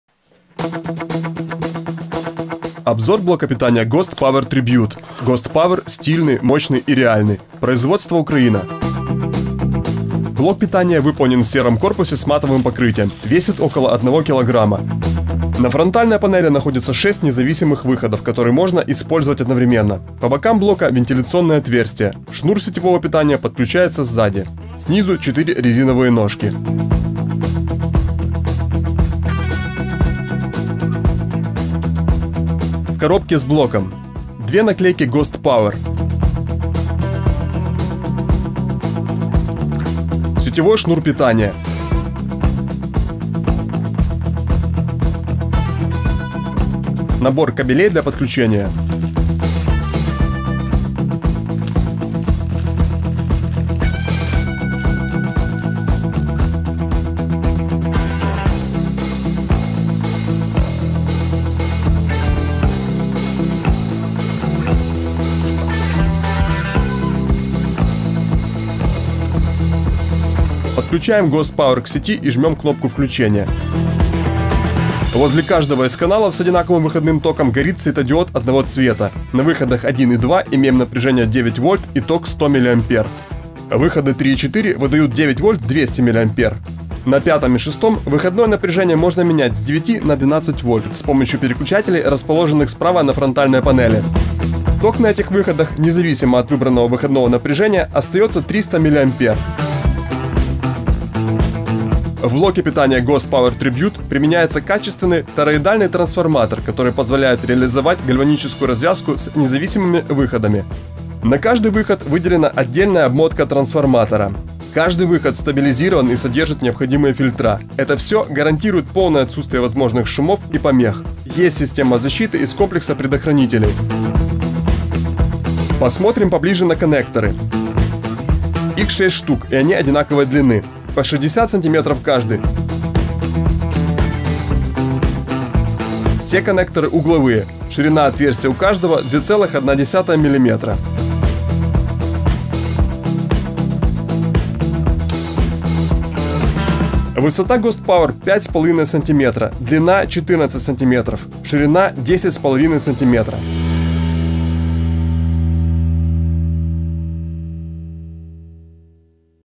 Блок питания Ghost Power Tribute
Обзор блока питания для педалей эффектов Ghost Power Tribute. Распаковка, внешний вид, размеры, вес. Режимы работы. Комплектация. Производство - Украина.